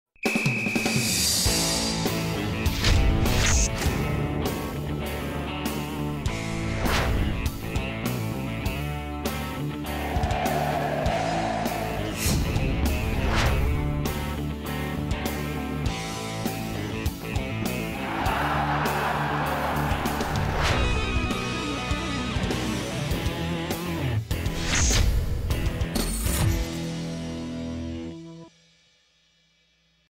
Teaser do Campeonato Sergipano de Futebol 2021 na Record
Gravado pela TV Atalaia/RecordTV no canal digital 8.1 na cidade de Aracaju/SE.